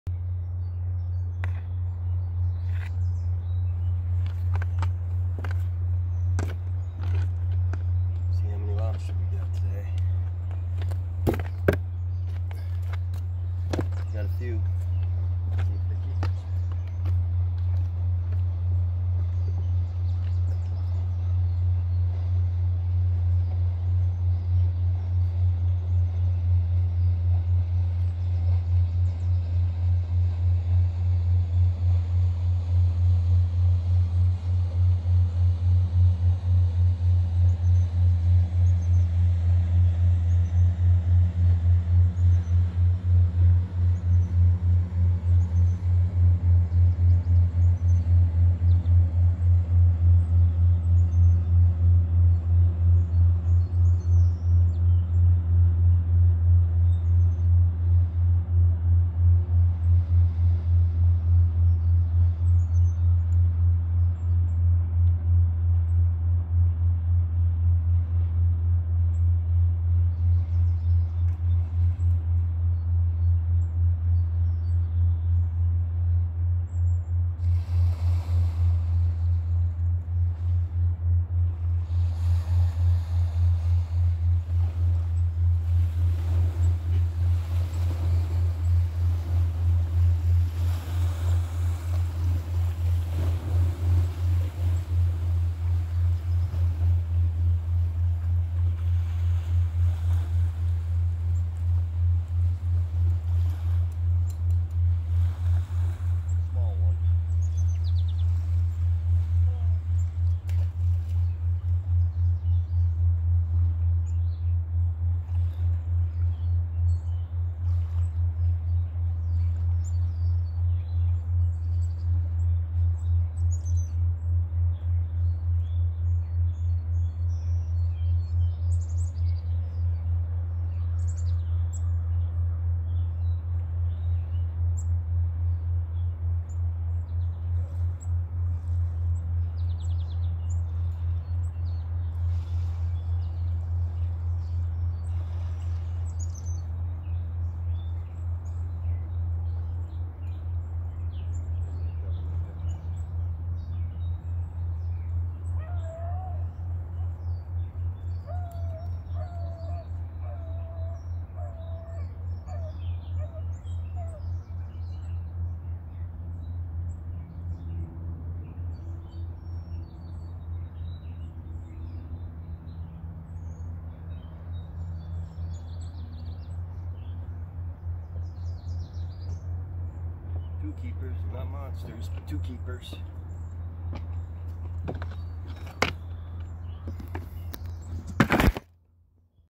|CATCHING LOBSTER IN THE CAPE COD CANAL| With Ditchfisher
Catching Lobster dinner in the Ditch. Cape cod canal never disappoints. Join me for adventures in the Ditch finding Monster Lobster , Striped Bass and Crab of all kinds. We will be testing out Crafty Lures a lure company with a focus on sharing the fun of lure design the lead designers are my young daughters.